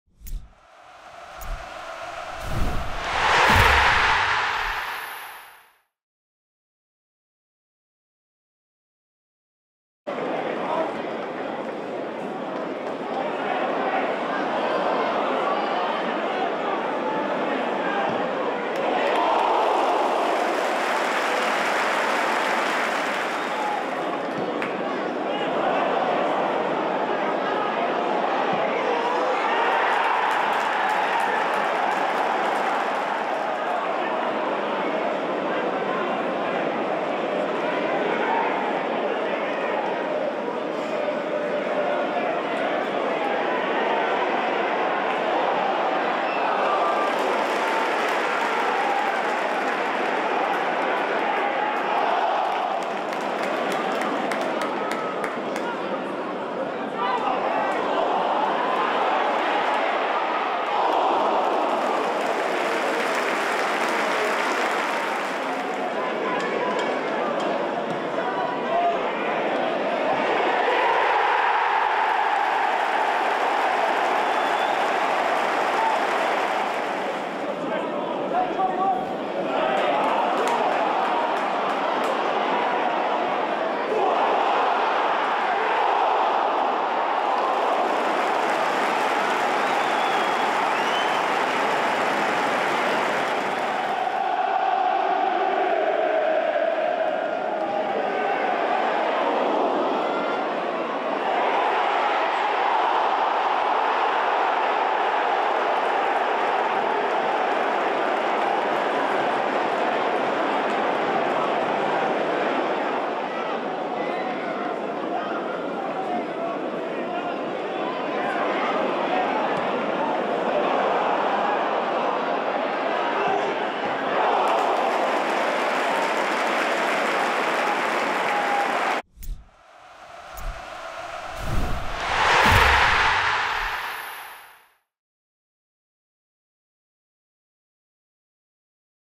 Highlights: Rotherham United 1-1 Reading (Sky Bet Championship) 20th October 2015 HD
Rotherham v Reading